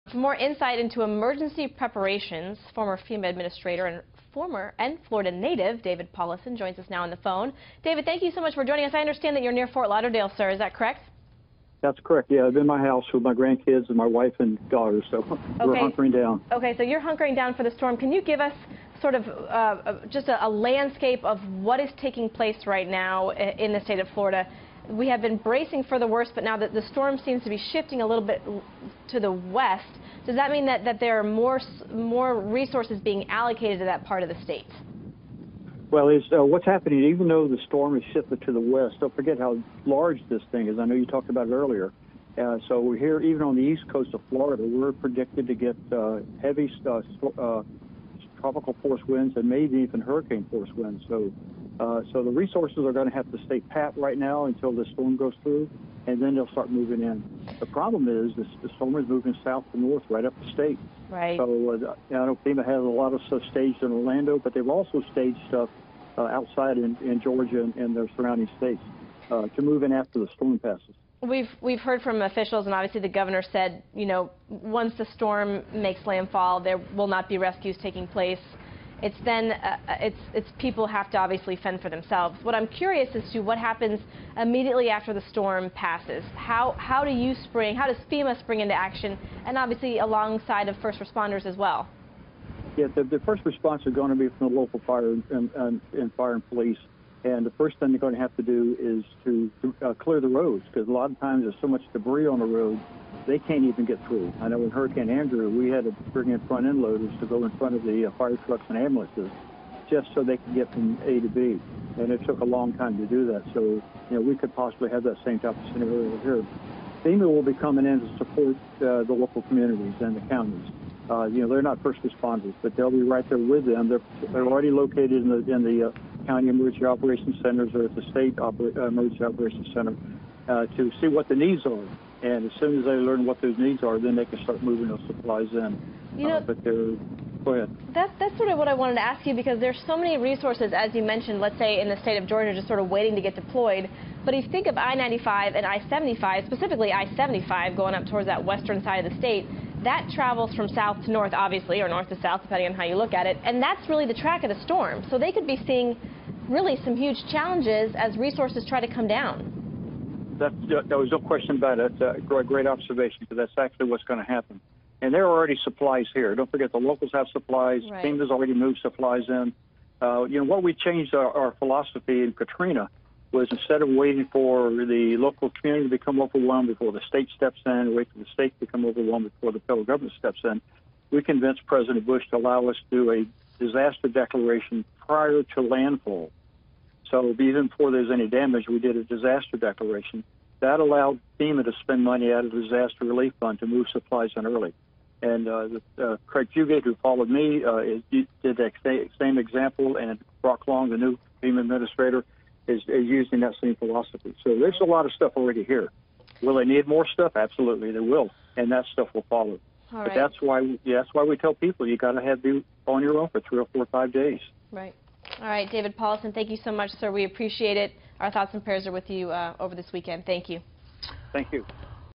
Former FEMA administrator on resources available to Florida
David Paulison provides insight as Florida braces for Hurricane Irma